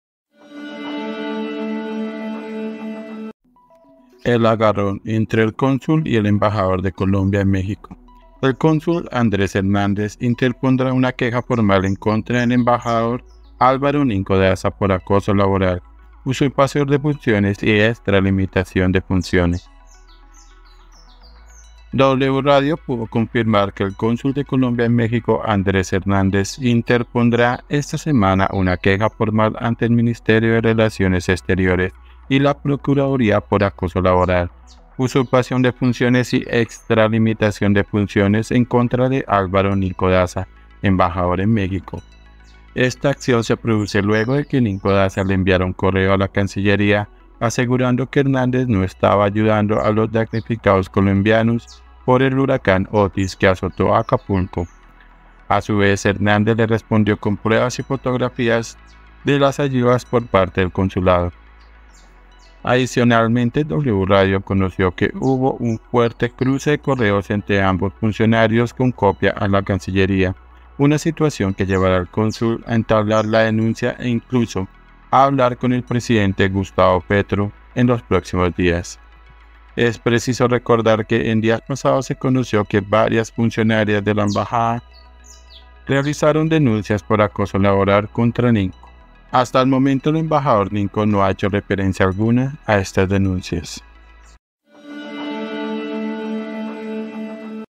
🎯 EL AGARRÓN ENTRE EL CONSÚL Y EMBAJADOR DE COLOMBIA EN MÉXICO
W Radio pudo confirmar que el cónsul de Colombia en México, Andrés Hernández, interpondrá esta semana una queja formal ante el Ministerio de Relaciones Exteriores y la Procuraduría por acoso laboral, usurpación de funciones y extralimitación de funciones, en contra de Álvaro Ninco Daza, embajador en México.
W RADIO